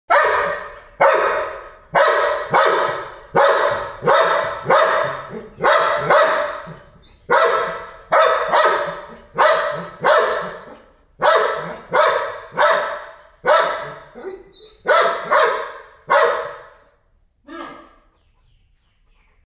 Dog barks at fish
Continuation of Bruno's reaction to pond fish